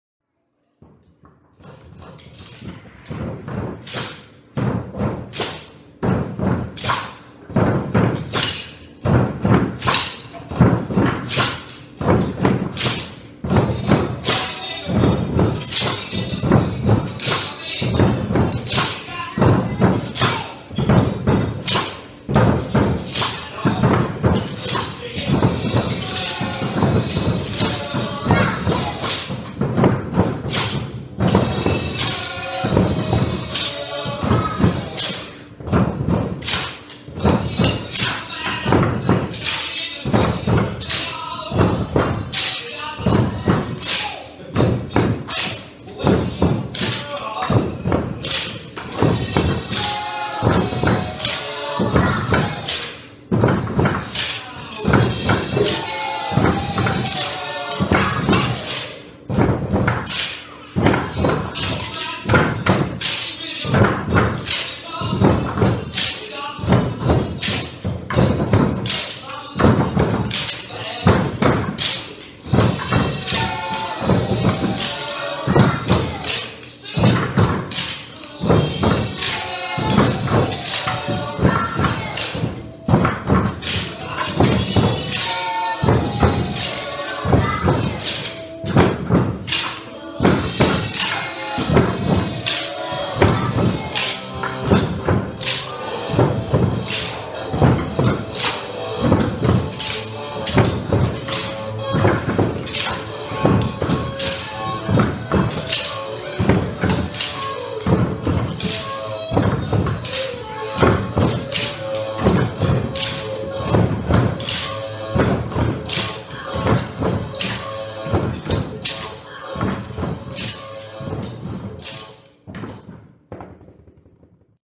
Mrs Zander's Rock and Rollers
We put our mad rhythm skills to the test this week on bucket drums. Enjoy!